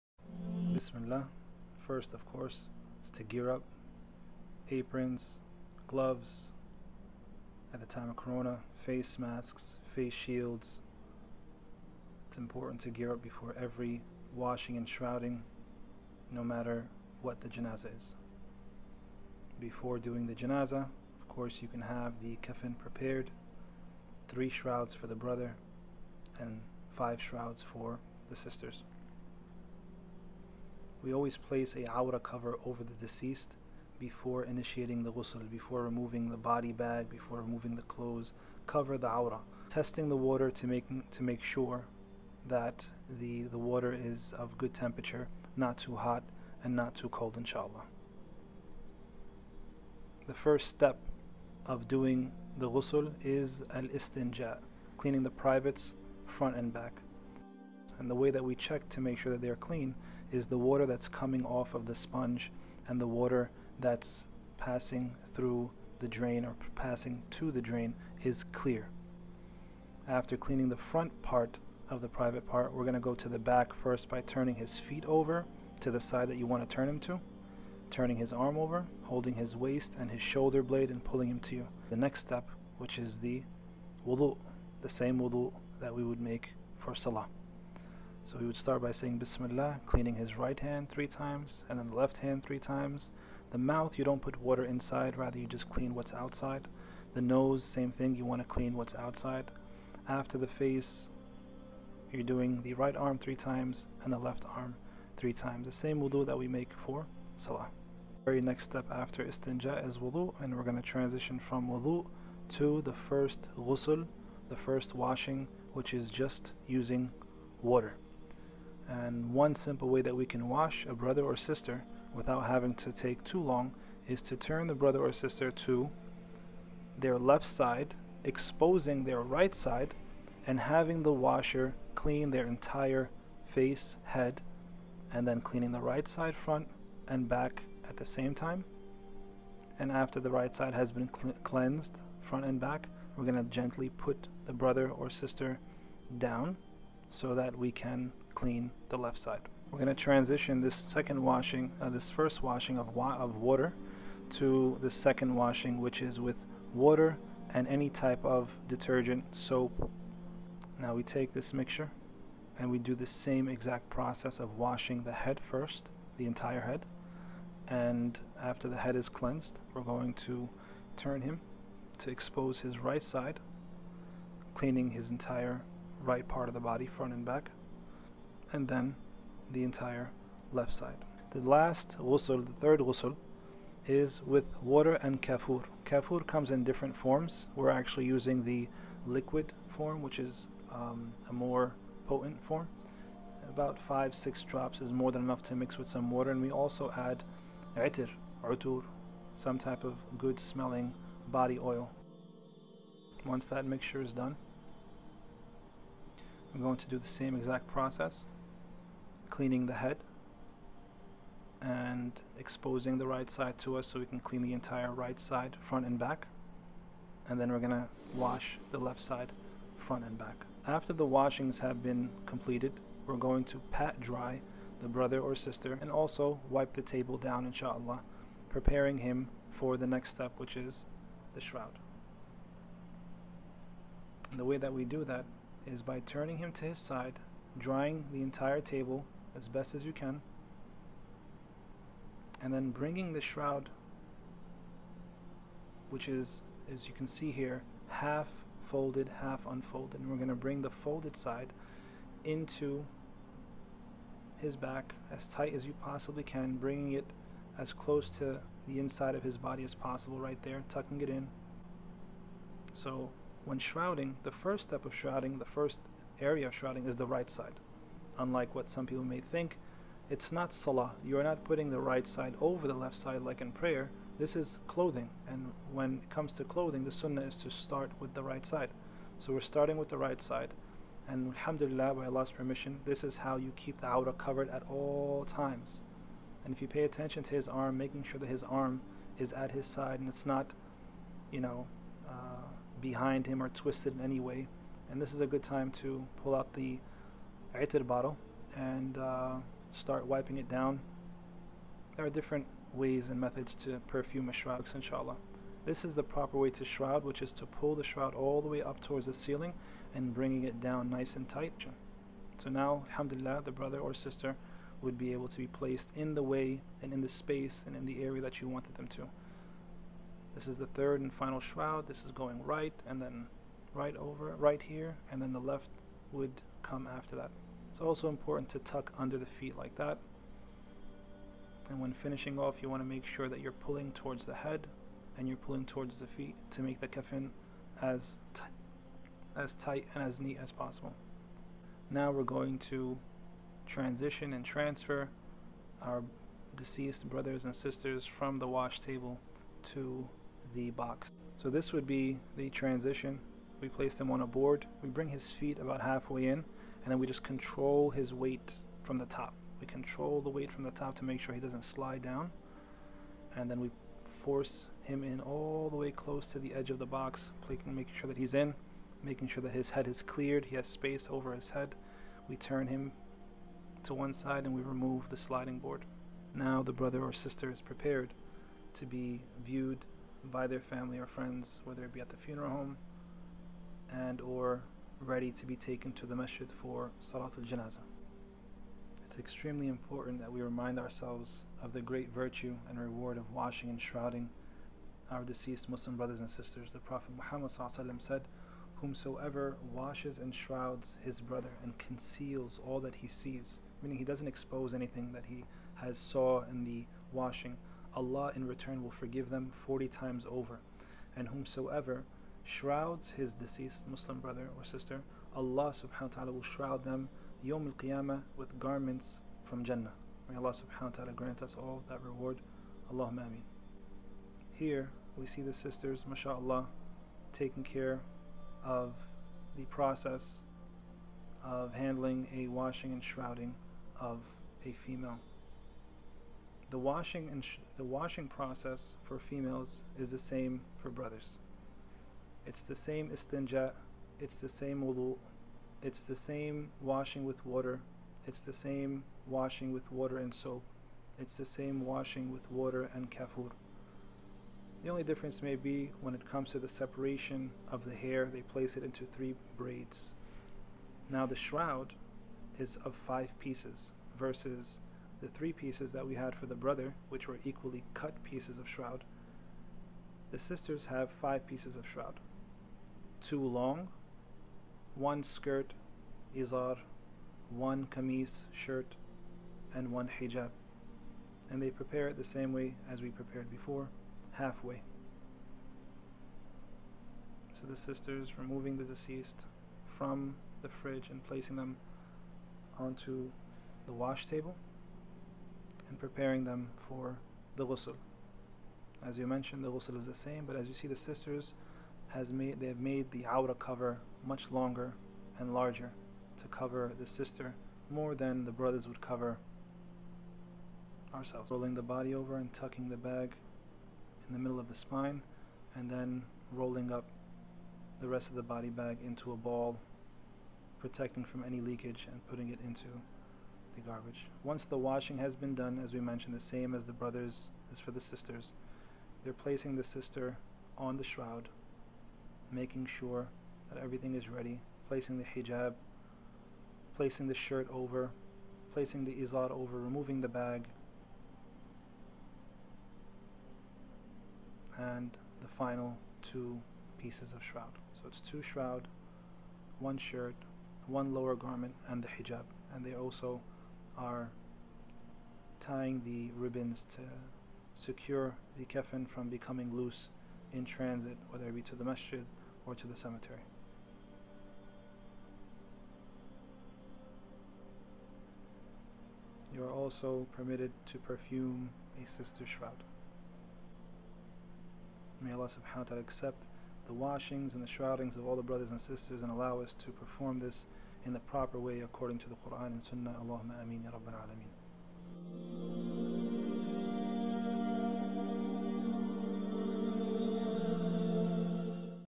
How to Pepare a Body in Islam Before Janazah (Men and Women) (Featuring Actors As Bodies)
- For resources on funeral and burial in the San Francisco Bay Area

This video was delivered at the Muslim Community Center - East Bay (MCC East Bay) in Pleasanton, California.